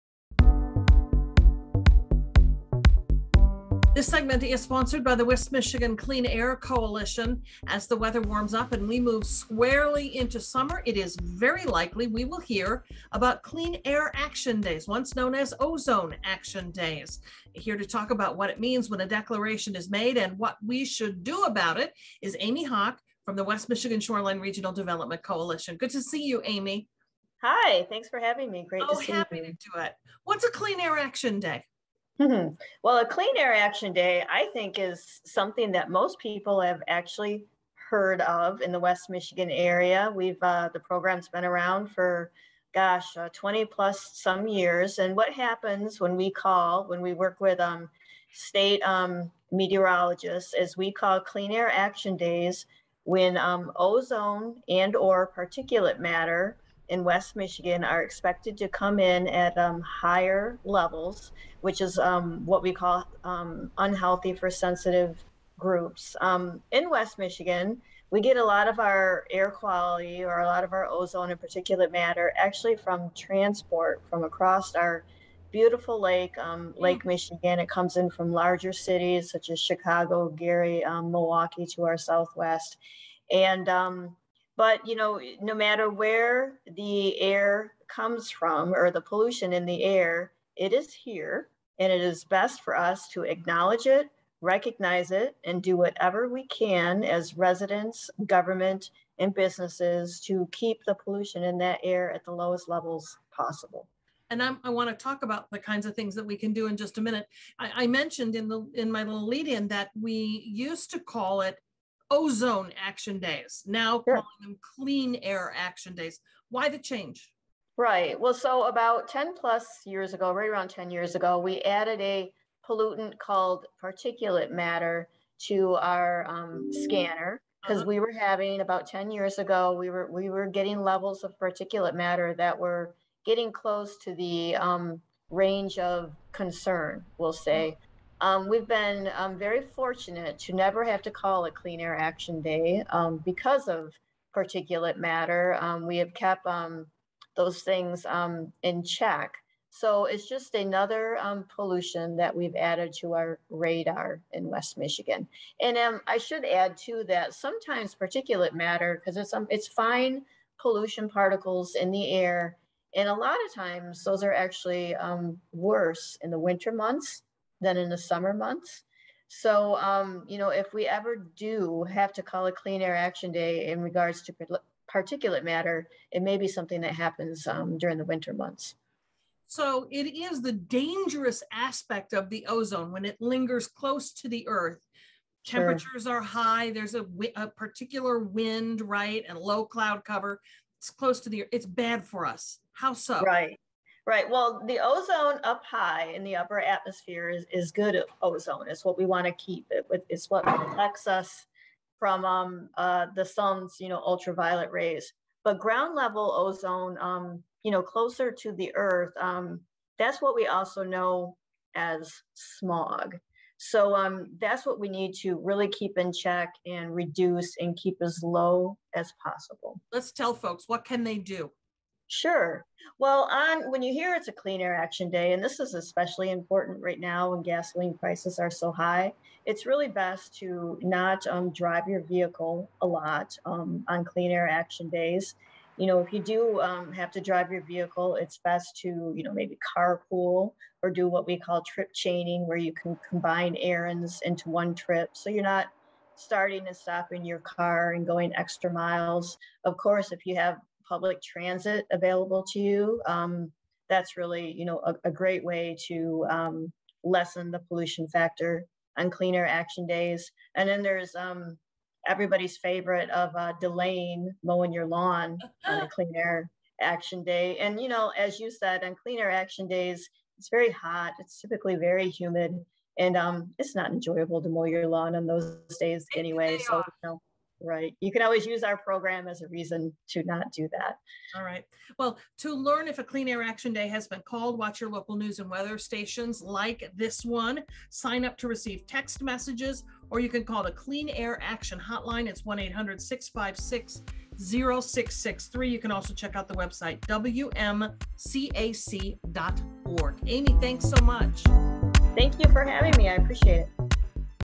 Pay attention to Clean Air Action Days when smog is at its worst
On these days, the public is asked to reduce emissions and protect their health.